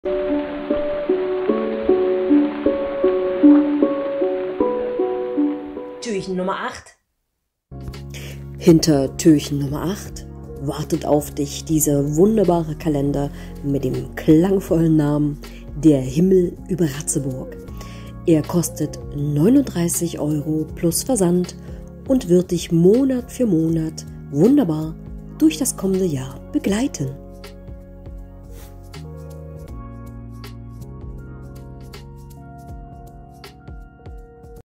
Adventskalender 2021
Türchen Nr. 8
Hinter Türchen Nr. 8 ist mein wunderbarer Kalender "der Himmel über Ratzeburg" versteckt. Er begleitet euch durch das kommende Jahr mit den Impressionen der Landschaft zwischen Ostsee und Elbe und dem Teil des grünen Bandes dieses stillen Landstrichs.

Advent Calendar 2021

Door No. 8
Behind door no. 8 is my wonderful calendar "the sky over Ratzeburg". He accompanies you through the coming year with the impressions of the landscape between the Baltic Sea and the Elbe and the part of the green belt of this quiet region.

"der Himmel über Ratzeburg"
30 × 30 cm/ 2021

39 Euro plus Versand/Plus Shipping

©Nadja Schüller-Ost/ 2021